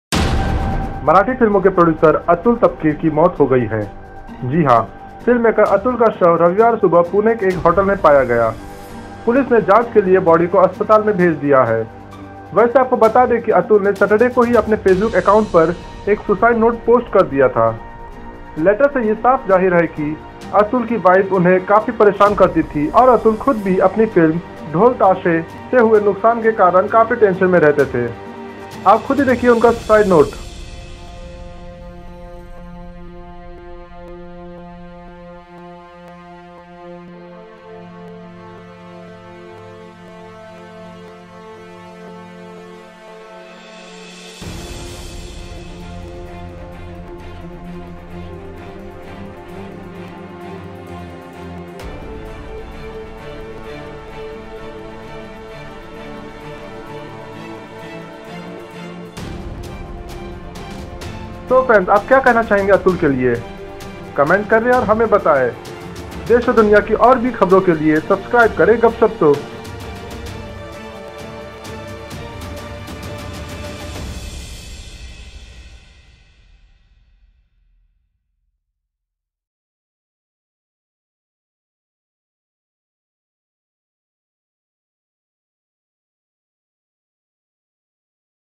Marathi Film Producer Atul B Tapkir Committed SUICIDE | Atul Tapkir Died leaving Suicide Note Video
Marathi film producer Atul B Tapkir was found dead at a luxury hotel in Pune on Sunday morning, police said but she was dead.

Check out the video and comment us your views.